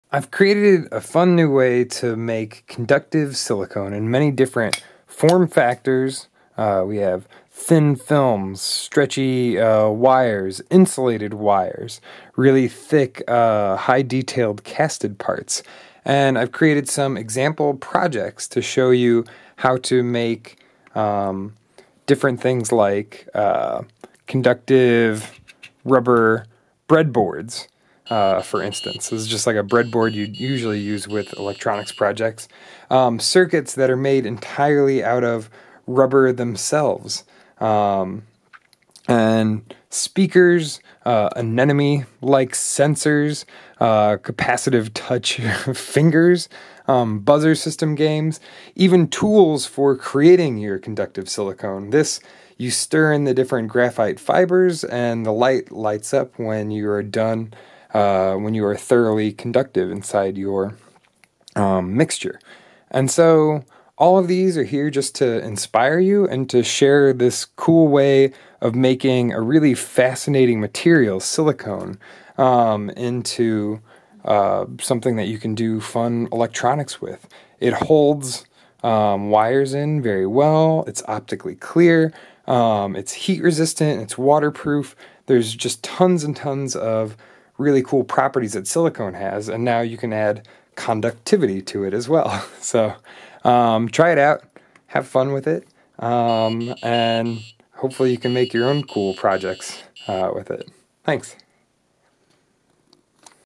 Silc Circuits: Intro to Conductive Silicone Rubber
Fun example projects to get you thinking about the possibilities of rubber circuits!